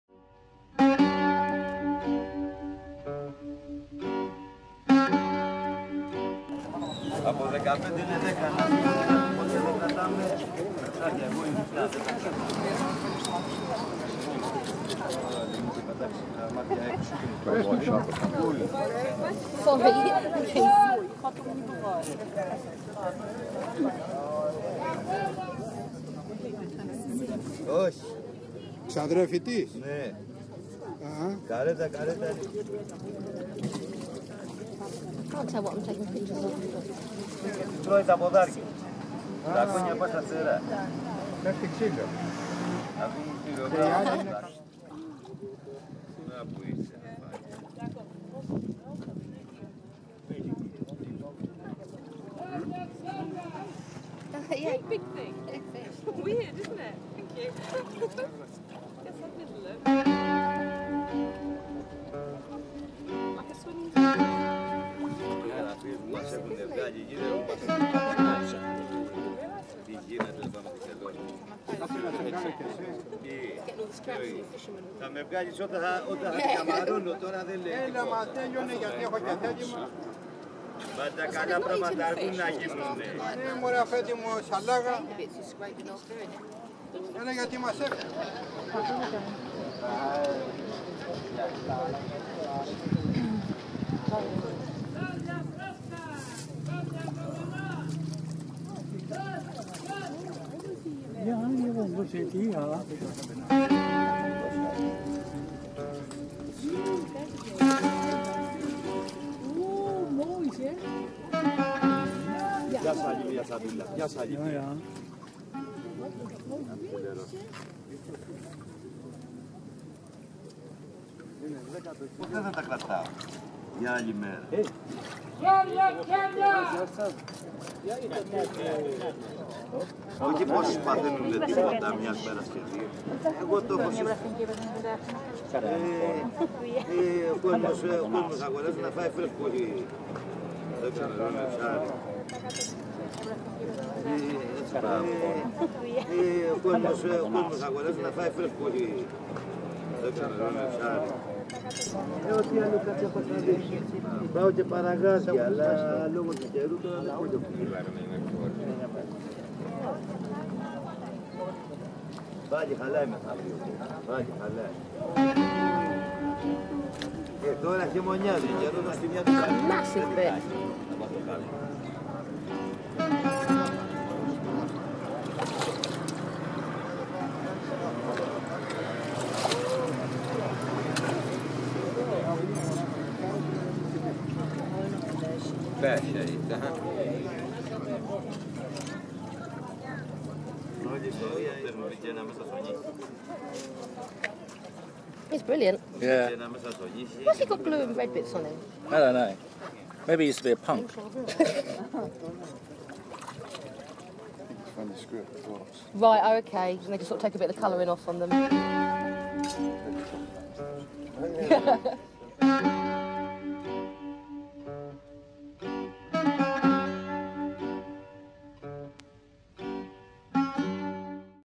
Kefalonia Turtle Time
Οι χελώνες της Κεφαλονιάς
Κουνώντας γάτες κανάλι
The Loggerhead sea turtle, Caretta caretta, filmed in Argostoli, Kefalonia.
The Loggerhead turtle is the only turtle known to nest in Greece. See three of them eating fishermen's scraps.